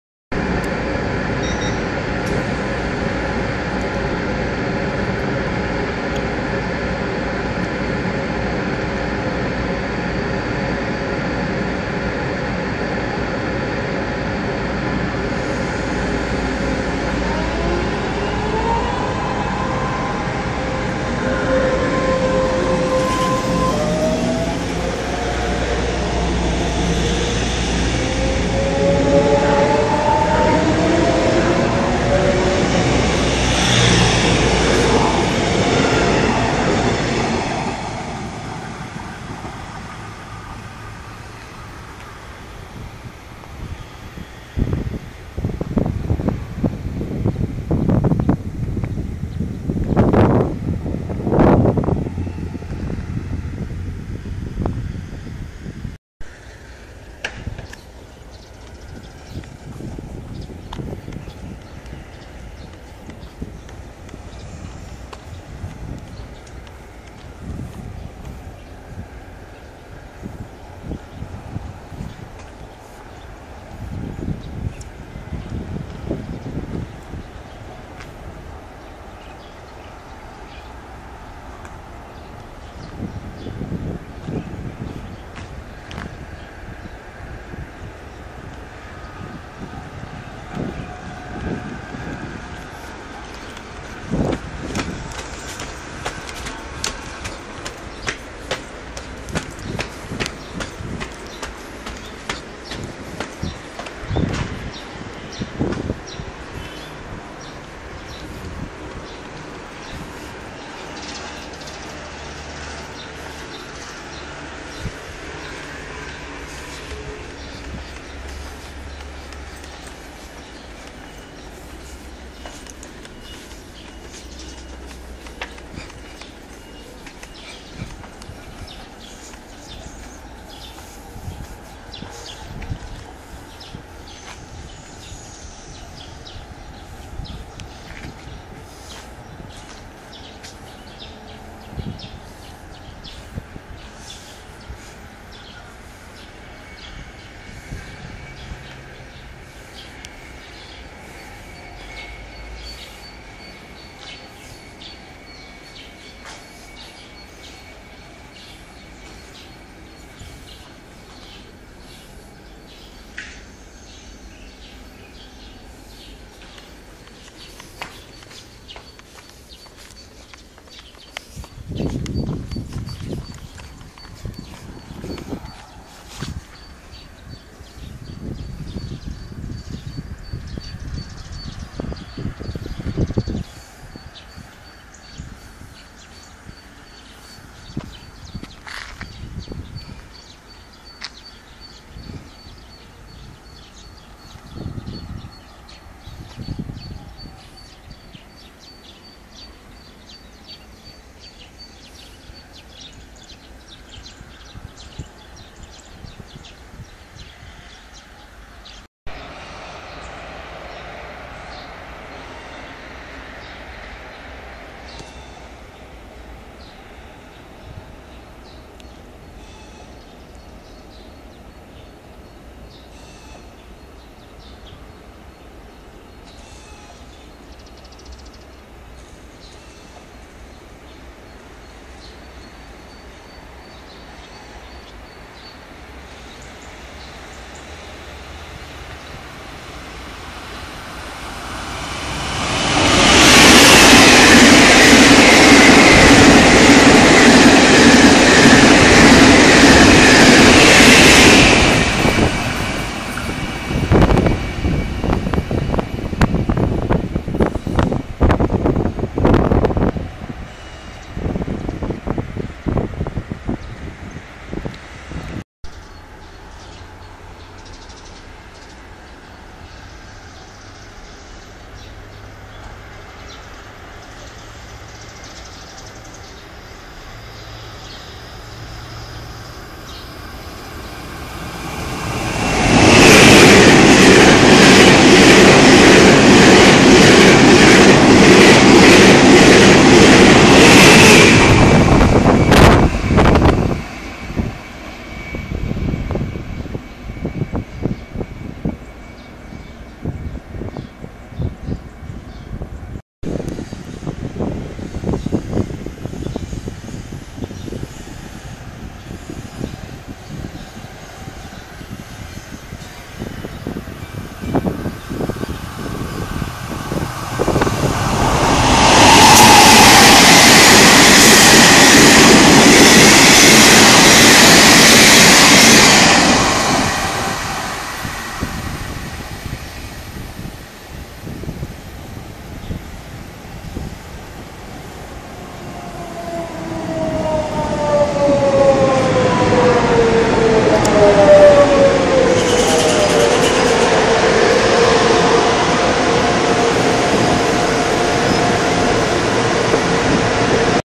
台鐵石龜站
斗南-大林間的小站,這次在白天攝影,也拍下數輛列車通過畫面